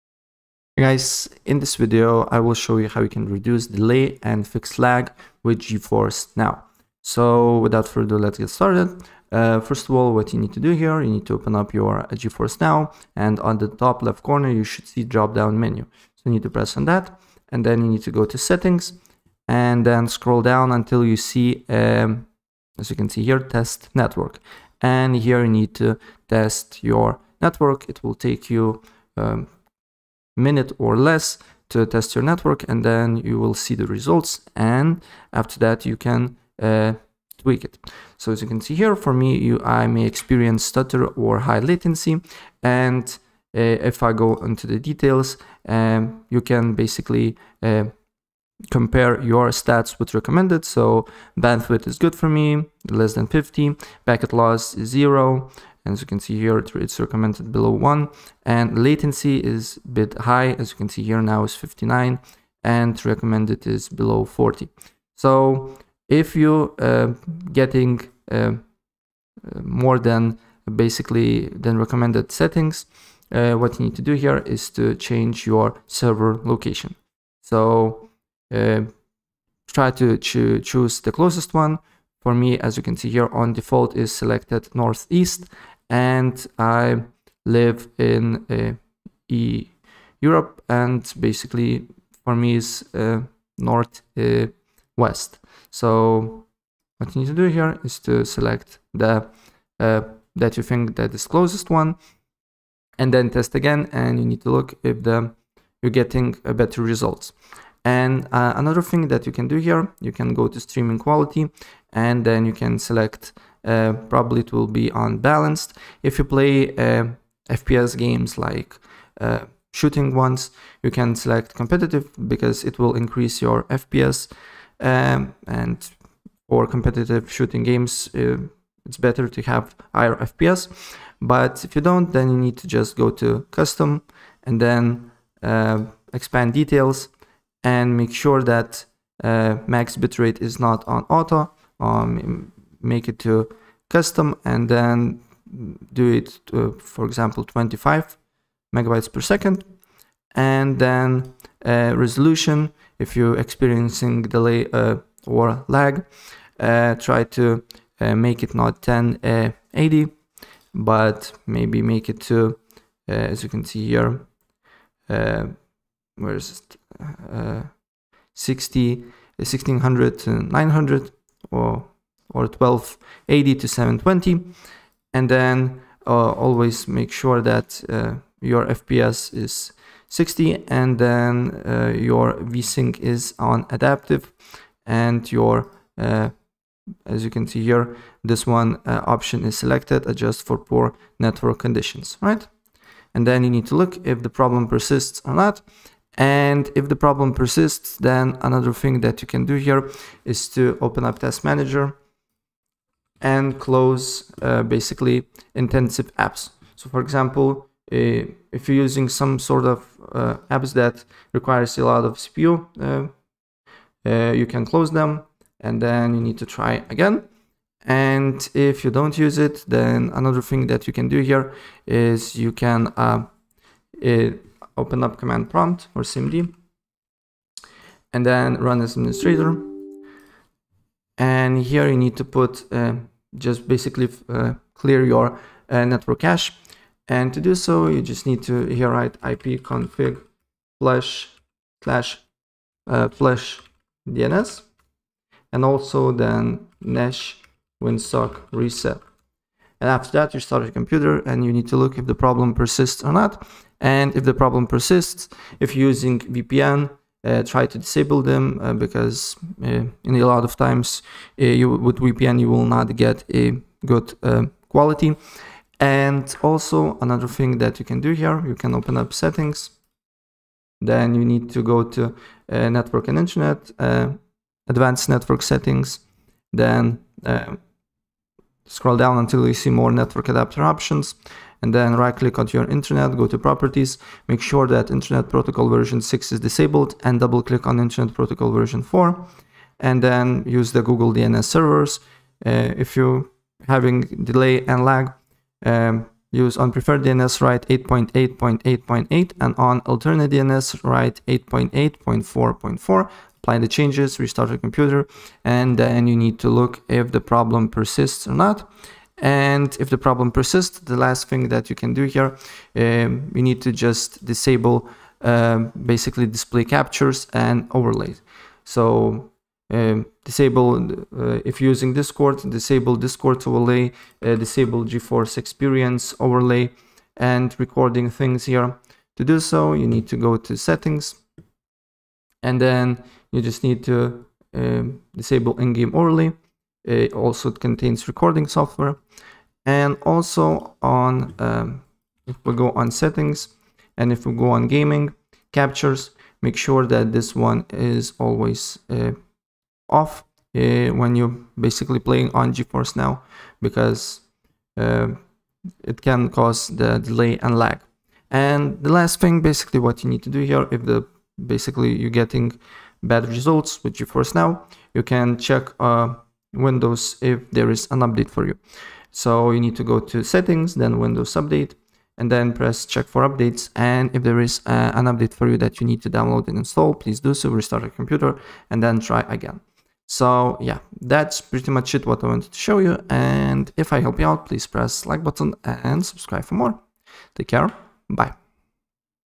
How To Reduce Delay & Fix Lag With Geforce Now
In this video I will show you How To Reduce Delay & Fix Lag With Geforce Now It's really easy and it will take you less than a minute to do it!